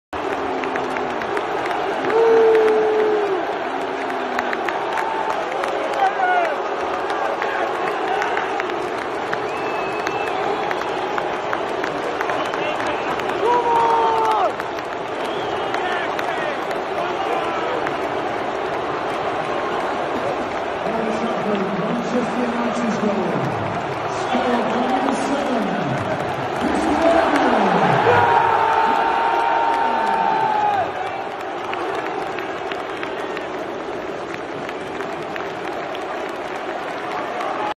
Cristiano Ronaldo receives a yellow card for taking off his shirt
Manchester United vs Villarreal
Champions League